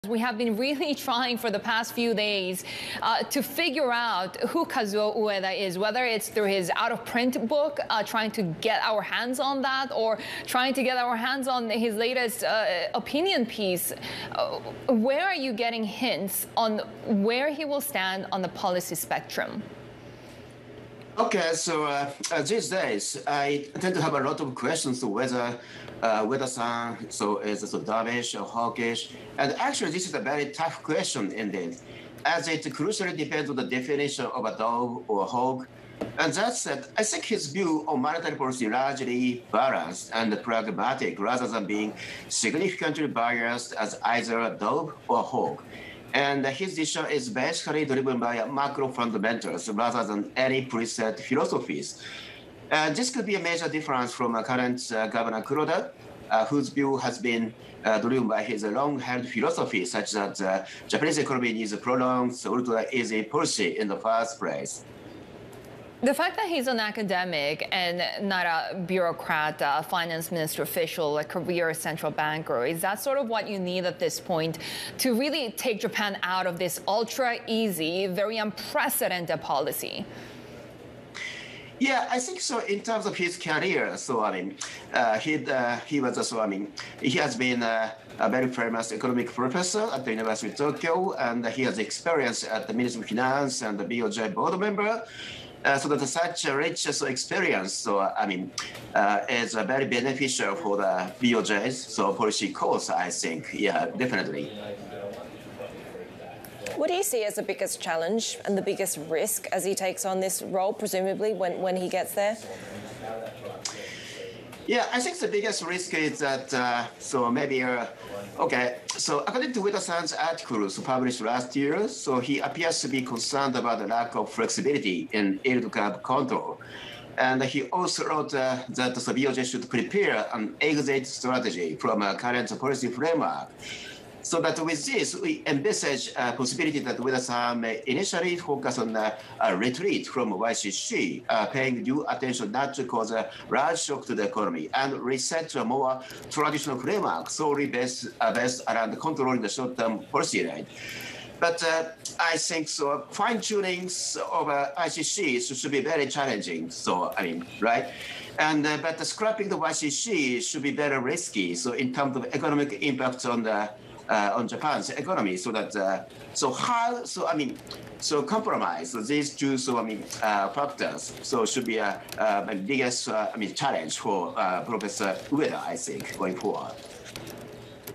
Ueda Is Neither Dovish Nor Hawkish: Goldman Sachs’s Baba
Naohiko Baba, chief Japan economist at Goldman Sachs, discusses the government naming Kazuo Ueda to head the BOJ, where he stands on policy and what could be his  biggest risk in the new role. Baba speaks on Bloomberg Television.